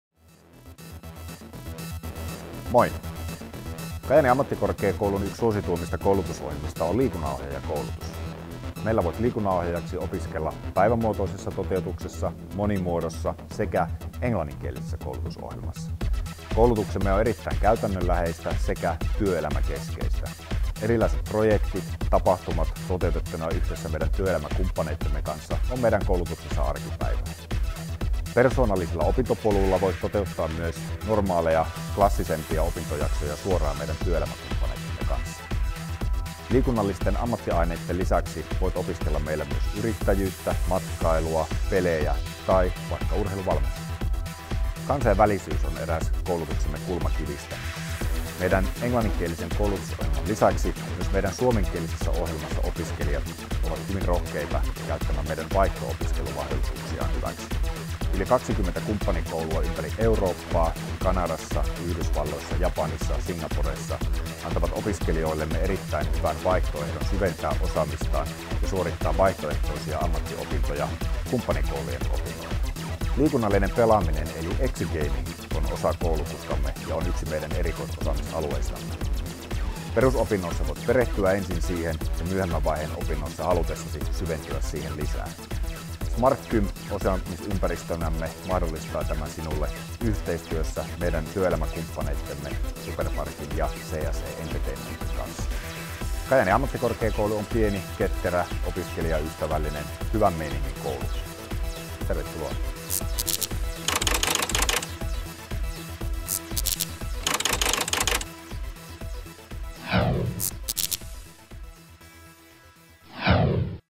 Opettajahaastattelu Kari Partanen opiskeluympäristö
Millaiset opiskeluympäristöt liikuntapuolelta löytyy Kajaanin ammattikorkeakoulusta? Entä mihin liikunnalliseen ammattiin voit valmistua? Liikunnan lehtori Kari Partanen valaisee asiaa.

What kind of study environments can be found on the sports side at Kajaani University of Applied Sciences? And what kind of sports profession can you graduate from? Kari Partanen, a lecturer in sports, sheds light on this. ENG SUB.